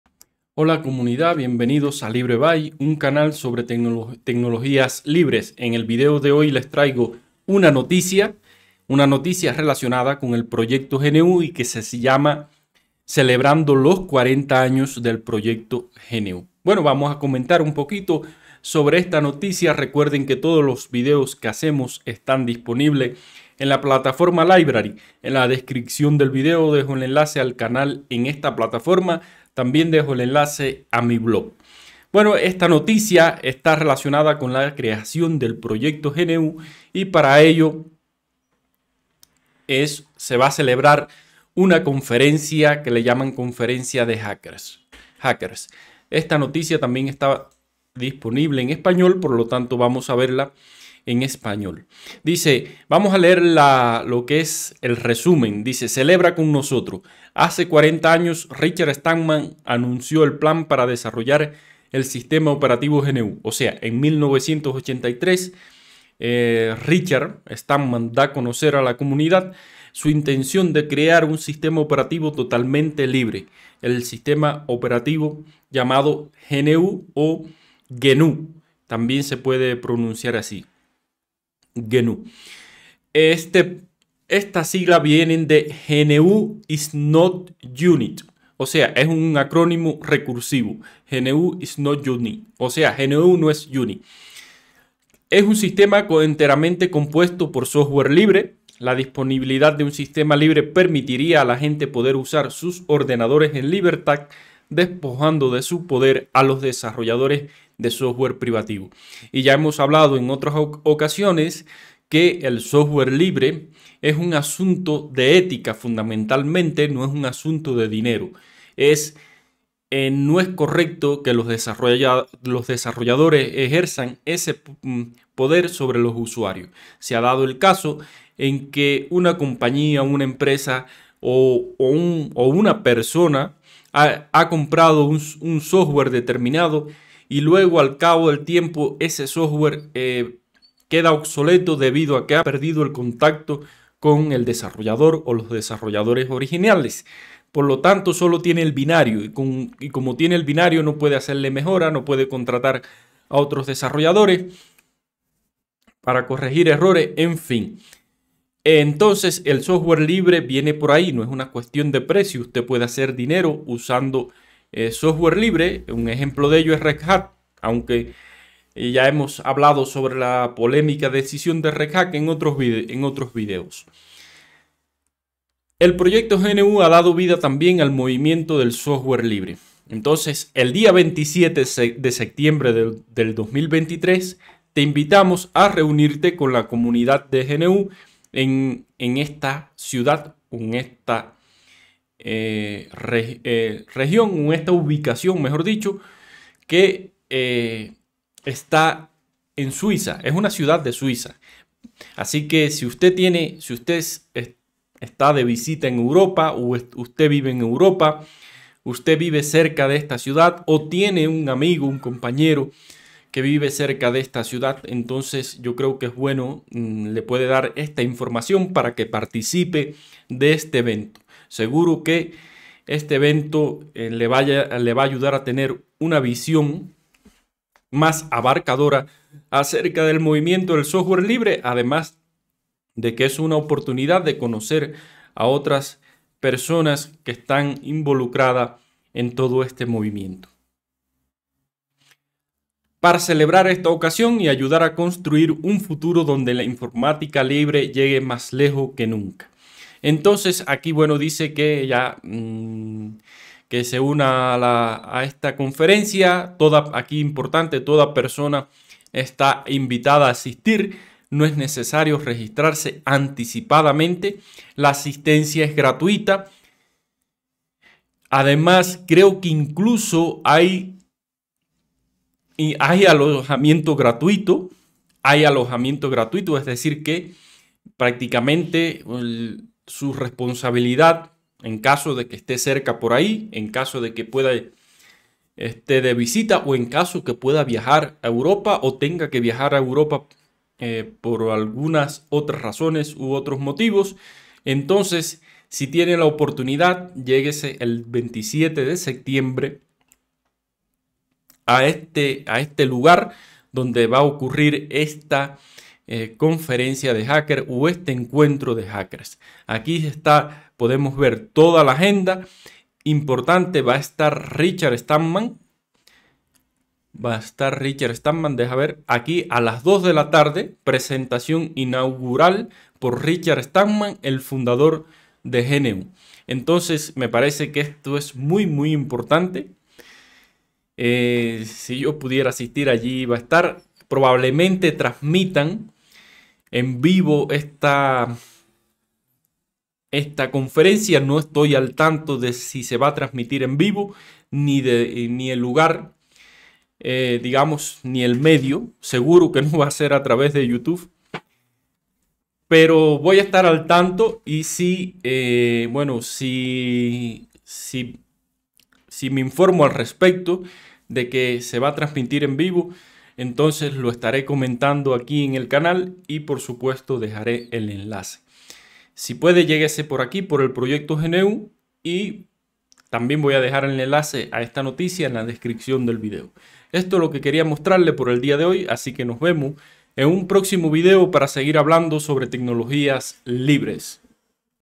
Celebrando los 40 años del poroyecto GNU
El día 27 de Septiembre de 2023 tienes la posibilidad de reunirte  con la comunidad de GNU en Biel/Bienne para celebrar los 40 años de la creación del proyecto GNU.